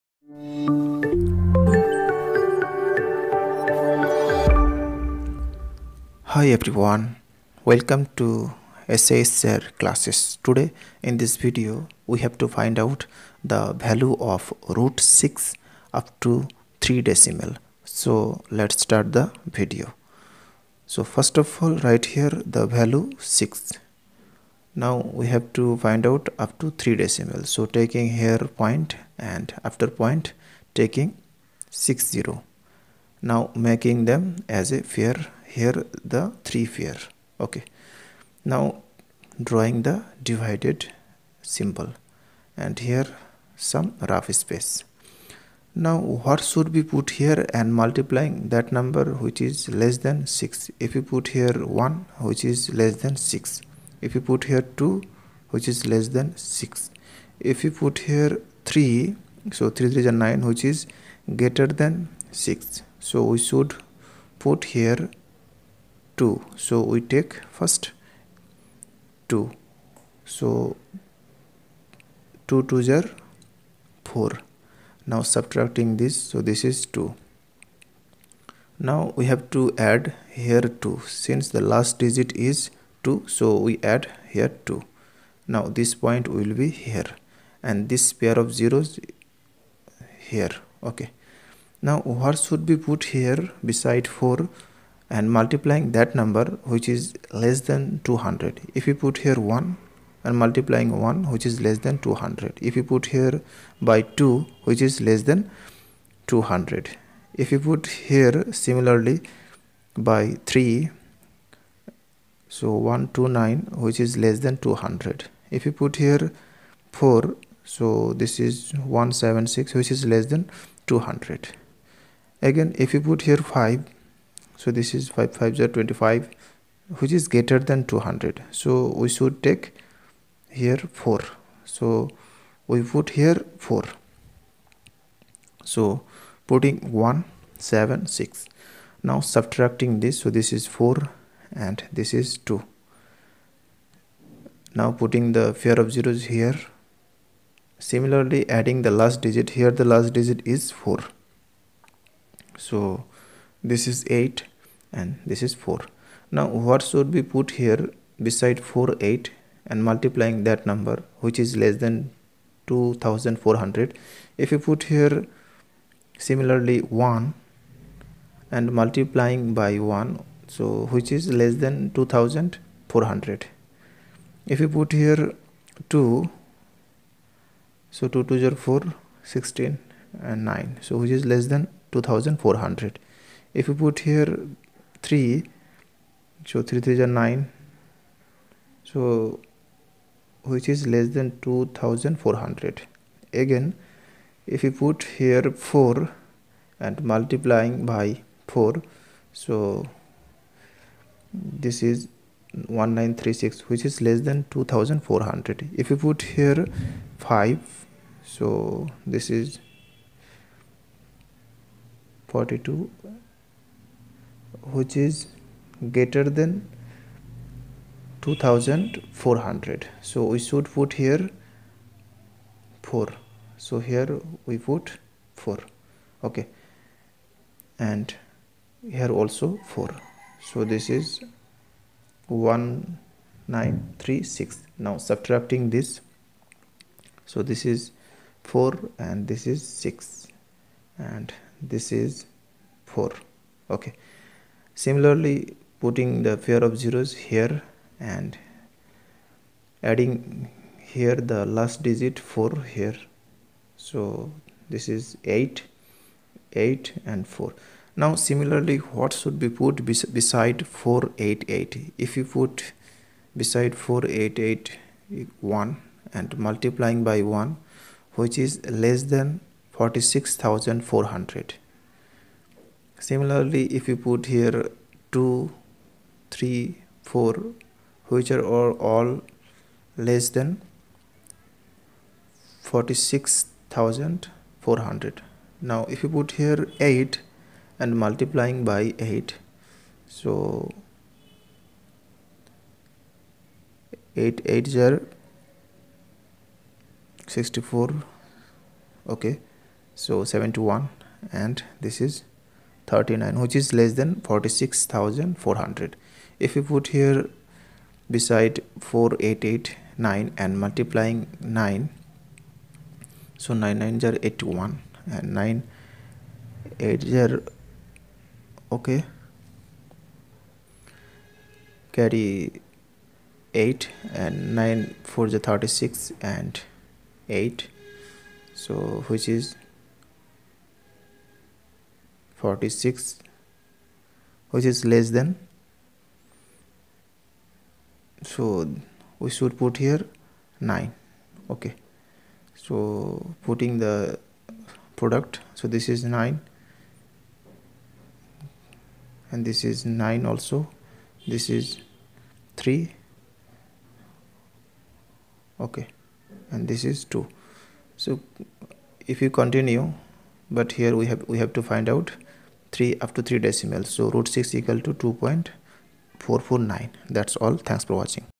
How to find the value of root 6 upto three decimal.
In Easy Way how to find the value of root 6 upto three decimal. Number System. @SHSIRCLASSES.